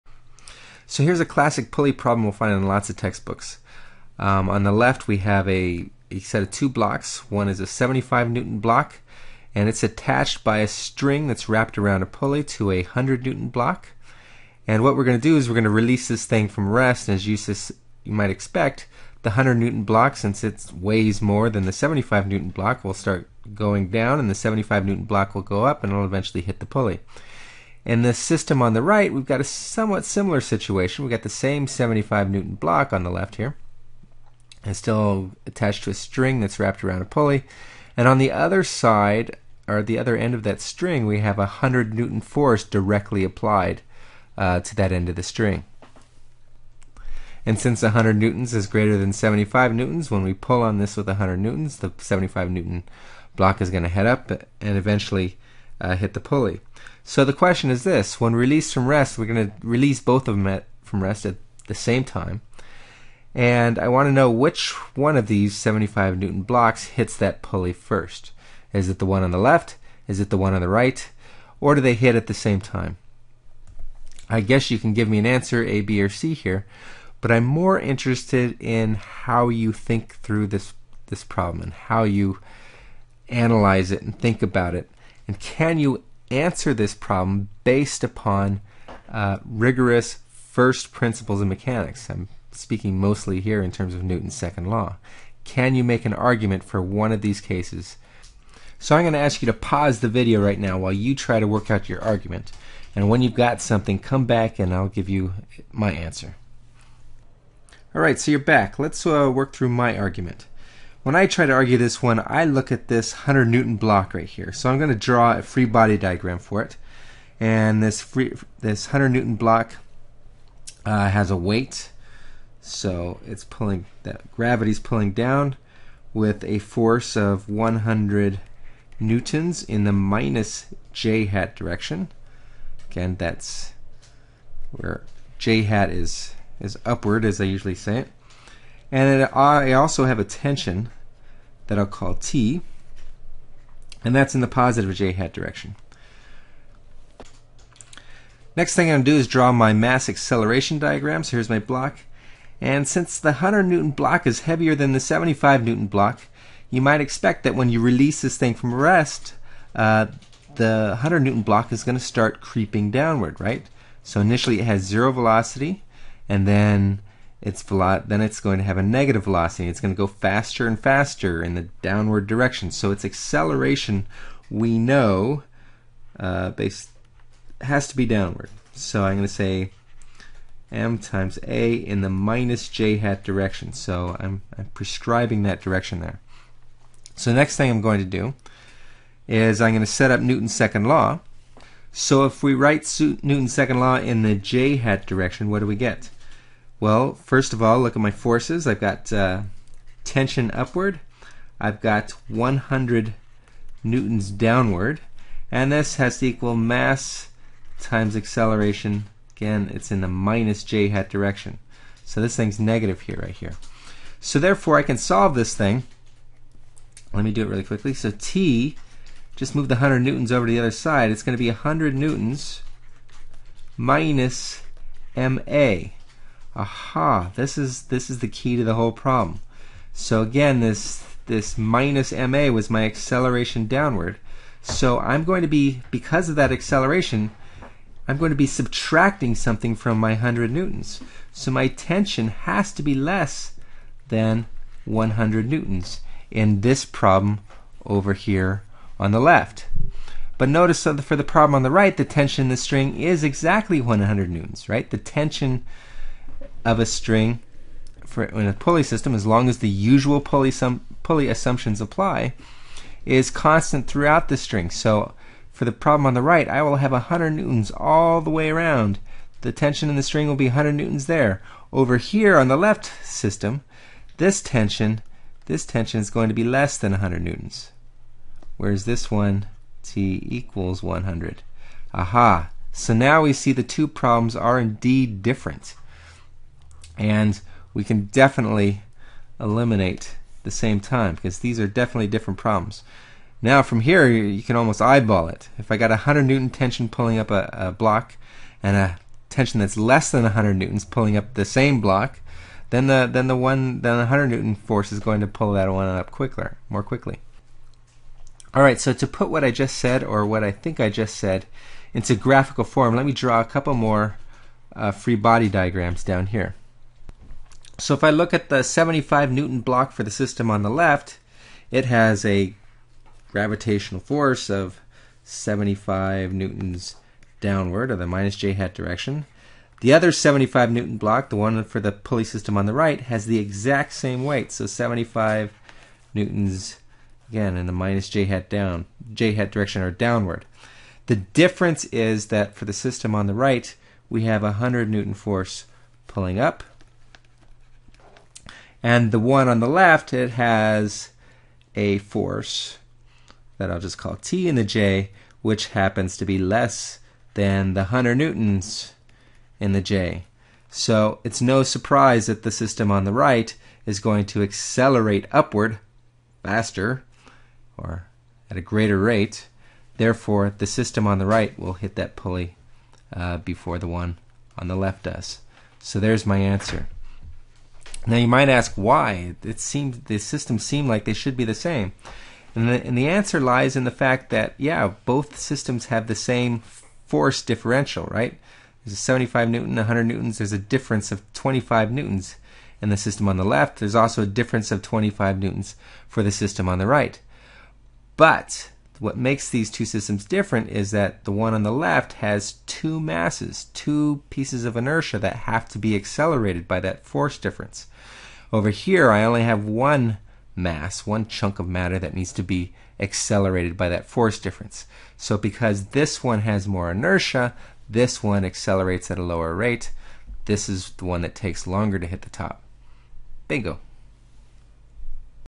Pulley Multiple Choice - Engineering Dynamics Notes & Problems
Here is a classic, but tricky pulley problem that tests your conceptual understanding of dynamics and inertia.